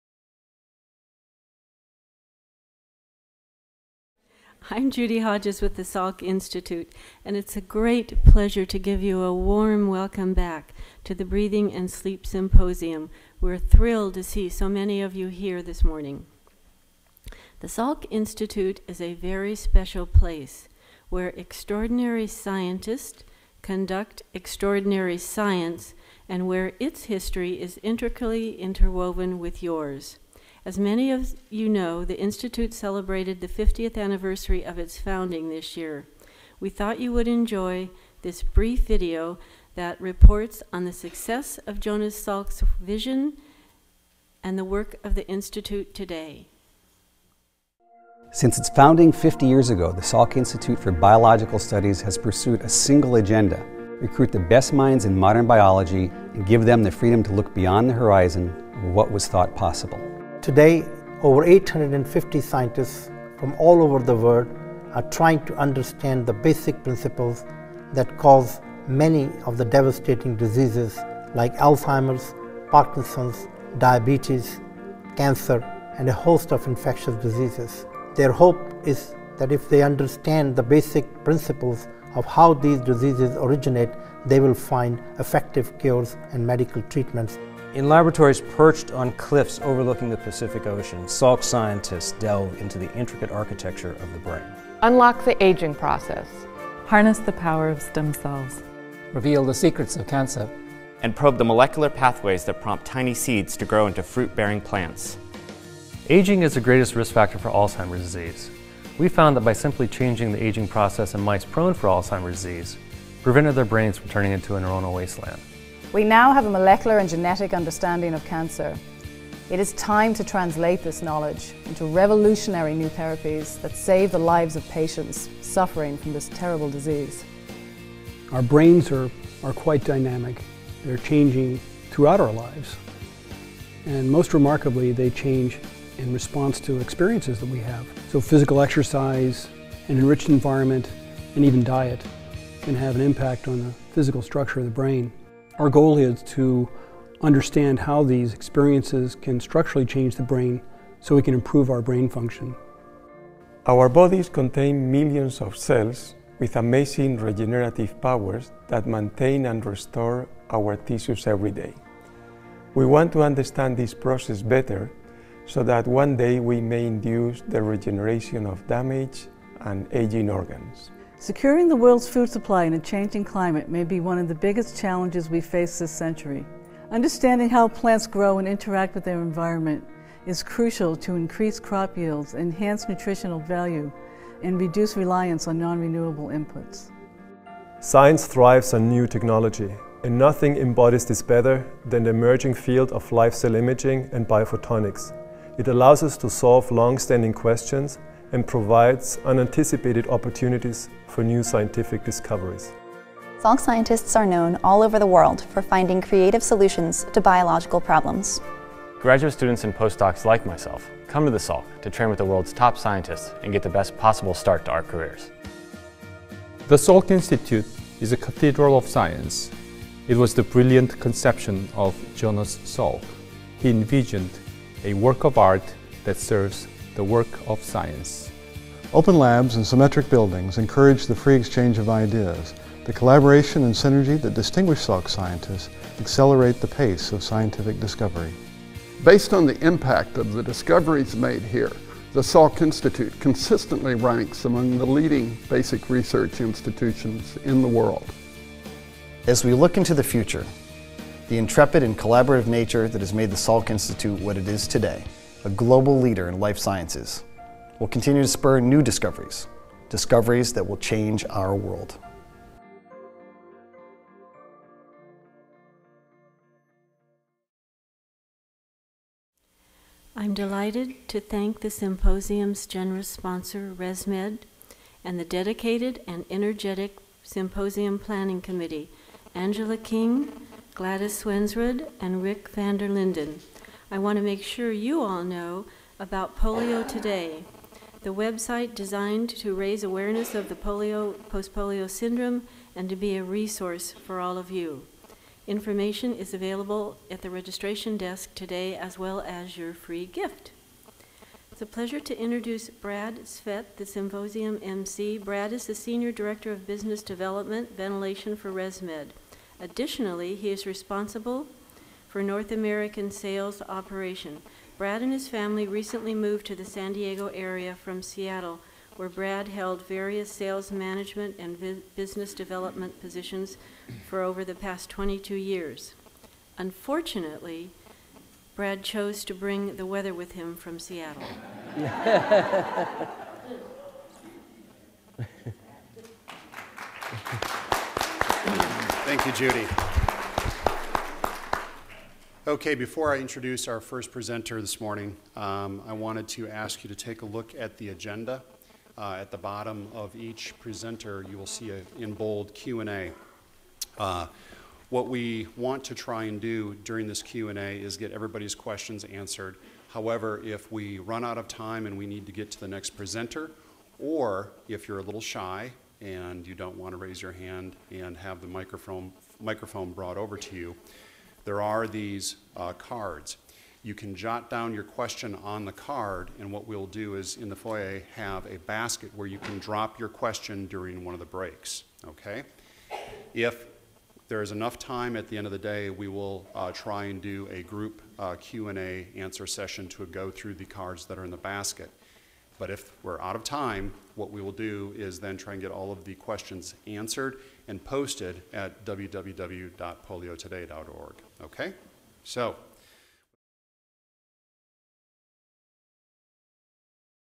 Judy Hodges, Salk Institute - Breathing & Sleep Symposium 2010
Welcome to the second annual Breathing & Sleep Symposium hosted by the Salk Institute of Biological Studies.